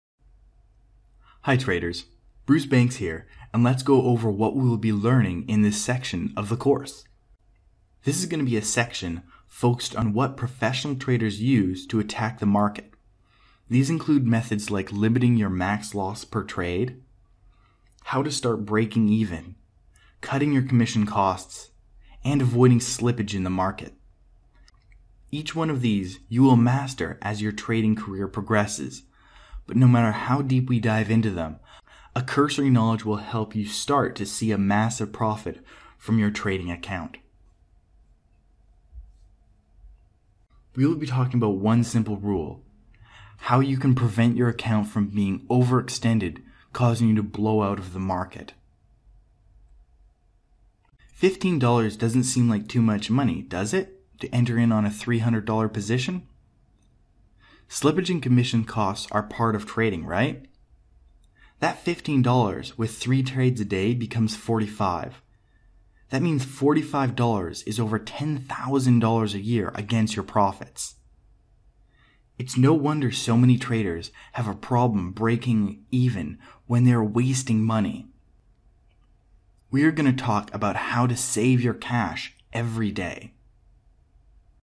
7 Intro To section on professional tricks
Bruce Banks: Trading Mindset, and Three Steps To Profitable Trading